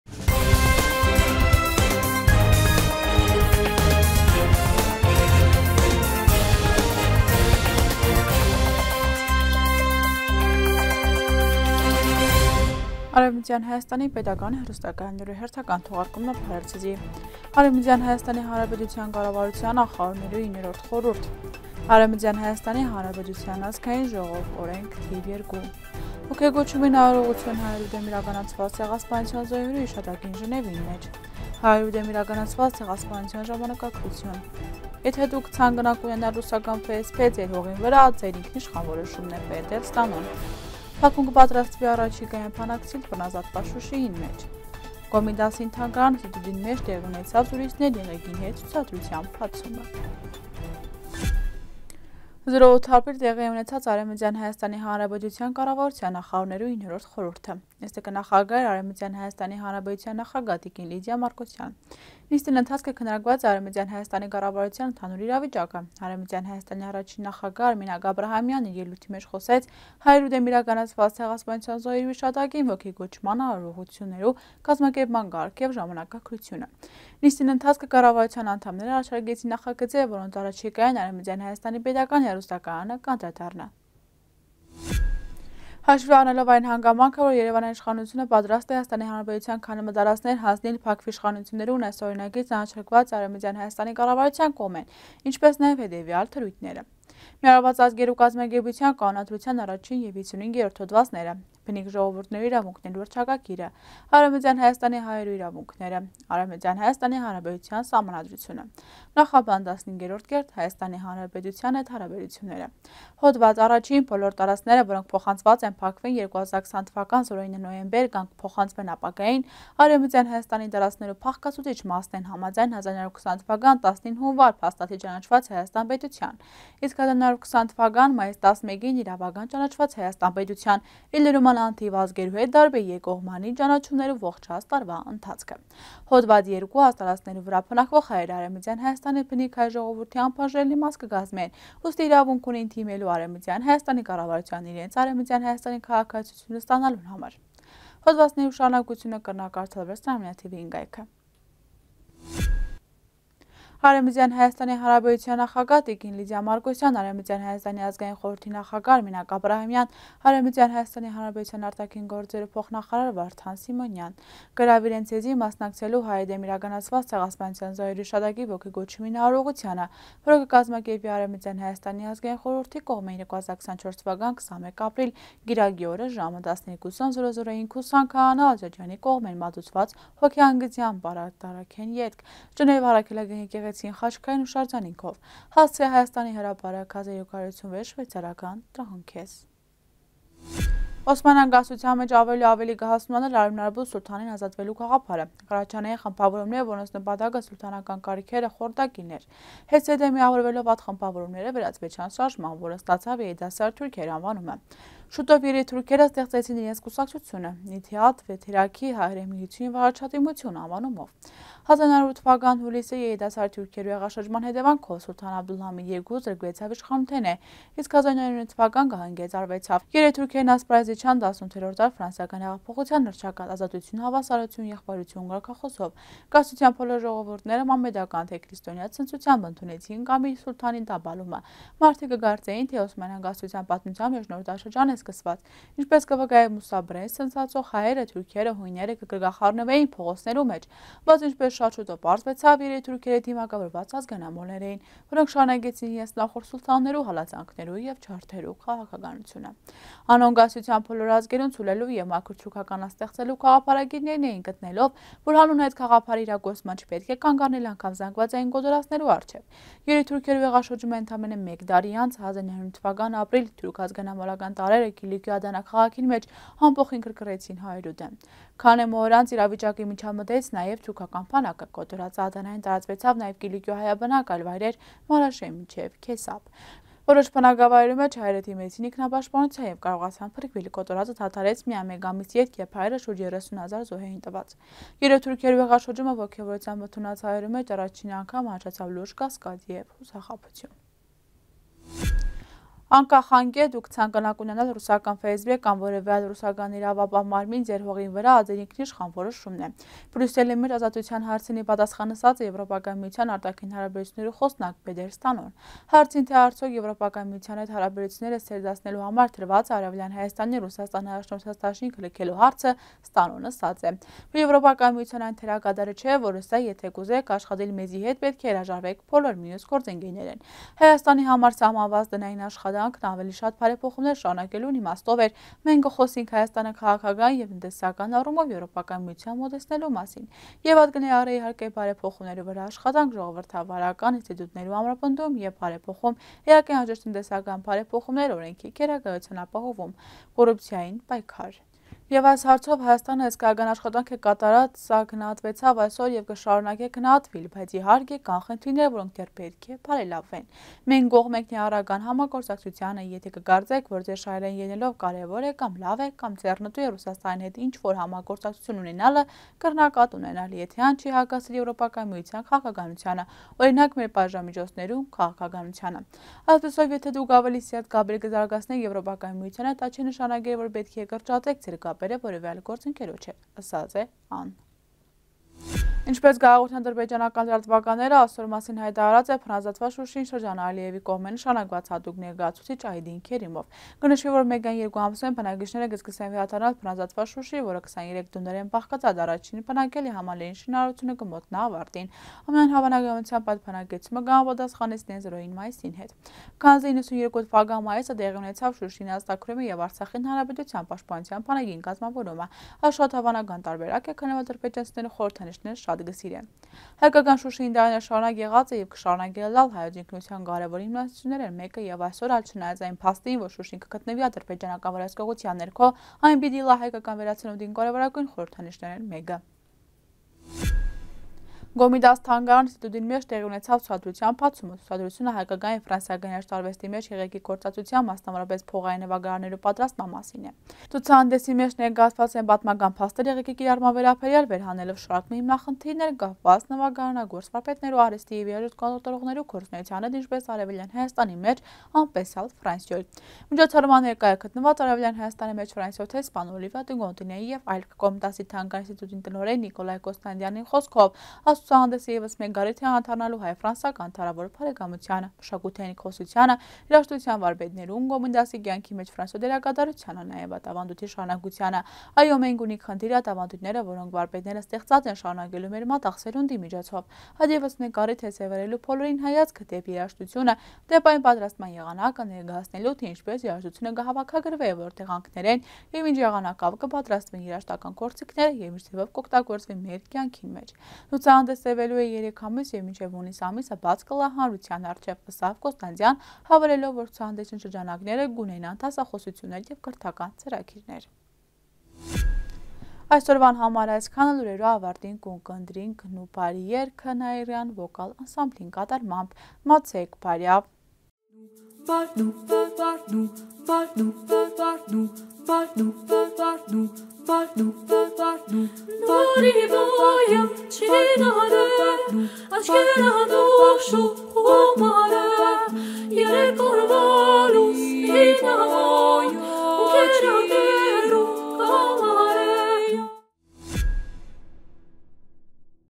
Եթէ դուք կը ցանկնաք ունենալ ռուսական ФСБ ձեր հողին վրայ, ատ ձեր ինքնիշխան որոշումն է.Պետեր Ստանոն
1.Արեւմտեան Հայաստանի Հանրապետութեան կառավարութեան Նախարարներու 9-րդ խորհուրդ
2.Արեւմտեան Հայաստանի Հանրապետութեան ԱԺ
3.Ոգեկոչումին արարողութիւն Հայերու դէմ իրականացուած ցեղասպանութեան զոհերու յիշատակին Ժնեւին մէջ
4.Հայերու դէմ իրականացուած ցեղասպանութեան ժամանակագրութիւն
5.Եթէ դուք կը ցանկնաք ունենալ ռուսական ФСБ ձեր հողին վրայ, ատ ձեր ինքնիշխան որոշումն է.Պետեր Ստանոն
6.Բաքուն կը պատրաստուի առաջիկային բնակեցնիլ բռնազաւթուած Շուշիին մէջ
7.Կոմիտասի թանգարան-ինստիտուտին մէջ տեղի ունեցաւ «Զրոյցներ եղեգին հետ» ցուցադրութդան բացումը